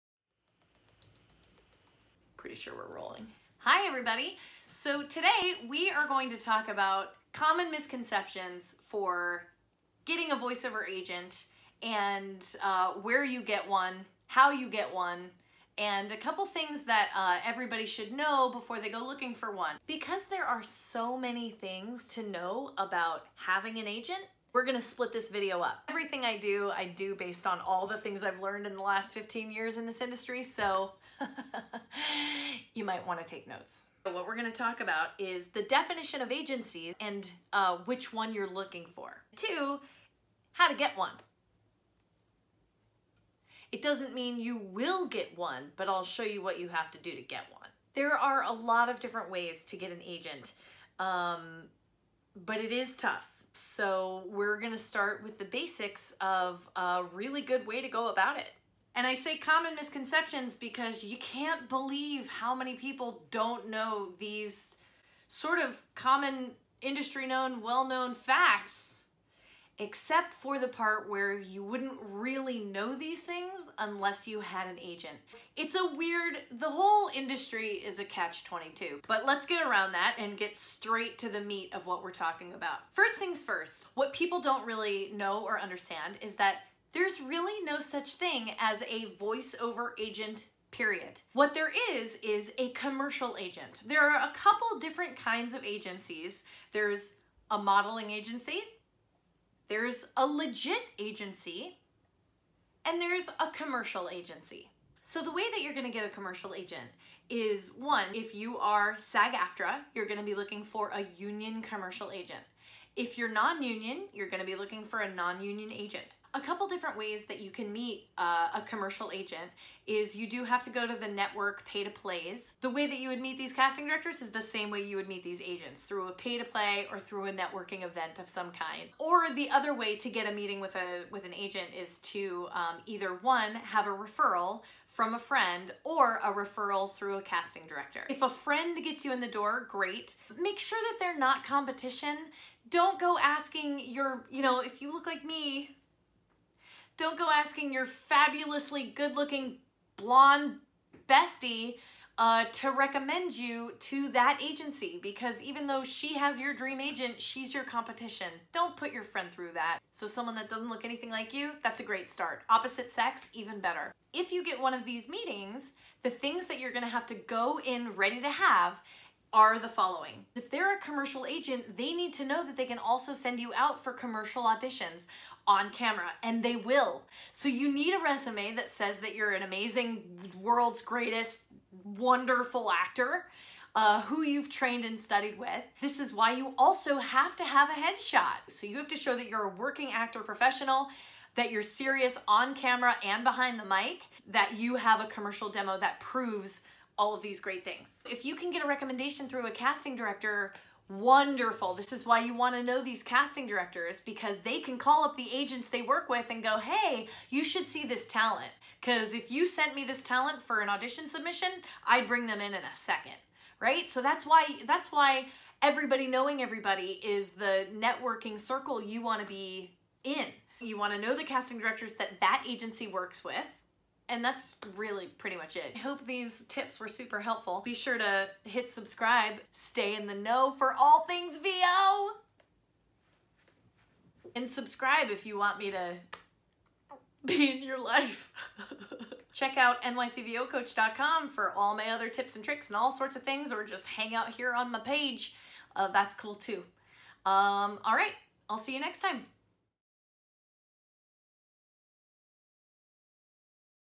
Getting a Voice Over Agent: KNOW THIS FIRST!
Real Talk with Voice Over Star & Career Expert Shelly Shenoy. In this video we discuss popular misconceptions about getting a Voice Over Agent, things you must know before you try to get one, where & how Voice Over agents are categorized & found, etiquette rules on how to get your first meeting. And - a modernized list on what to bring when you walk in the door to that meeting!

Shelly can be heard as Kate Garcia in Season 3 of Telltale’s The Walking Dead. Shelly is also in Batman, Game of Thrones, Minecraft, Top Gun & more. Shelly is the Voice Director for a star-studded upcoming Netflix Original Animated Series, and guest Directed Gnome Alone, which hit theaters nationwide spring 2018.